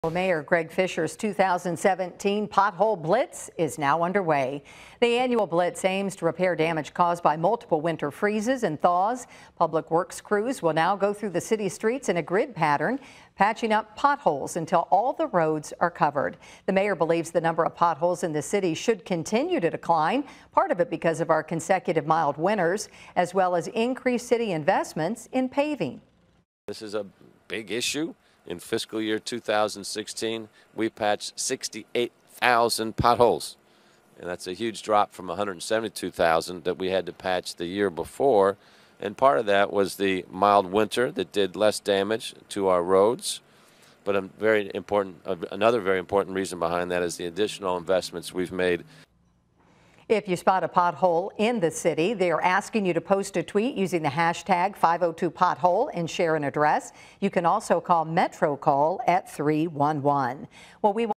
Mayor launches pothole blitz
Mayor Greg Fischer's 2017 Pothole blitz is now underway.